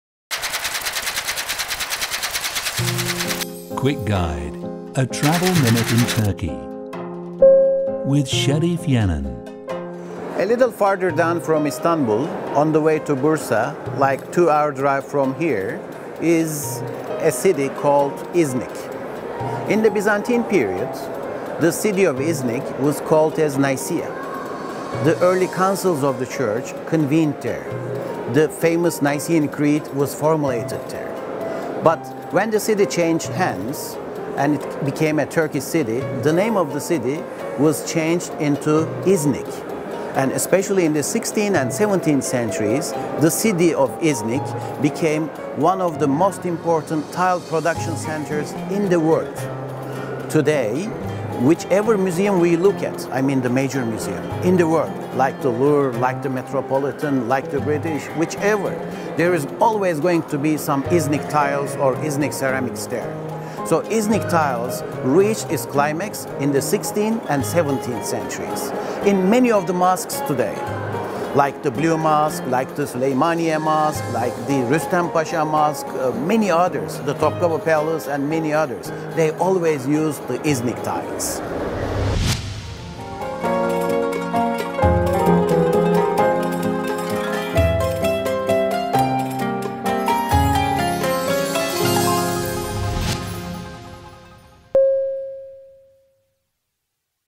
Quick Guide 16: Iznik Tiles
Nicaea was an important city of the Hellenistic and Roman Kingdom of Bithynia. In the Turkish period, the city was named as İznik and it became the largest tile production centre.

There were approximately 340 ateliers of tiles in İznik. When an Ottoman sultan wanted to build a new building, he sent a message to the governor of İznik. All the work was distributed to the ateliers. In the beginning of the 16th century, motifs on tiles had blue, dark blue and yellow colors on white background. In the second half of the century more motifs were used and color combination becomes more complex.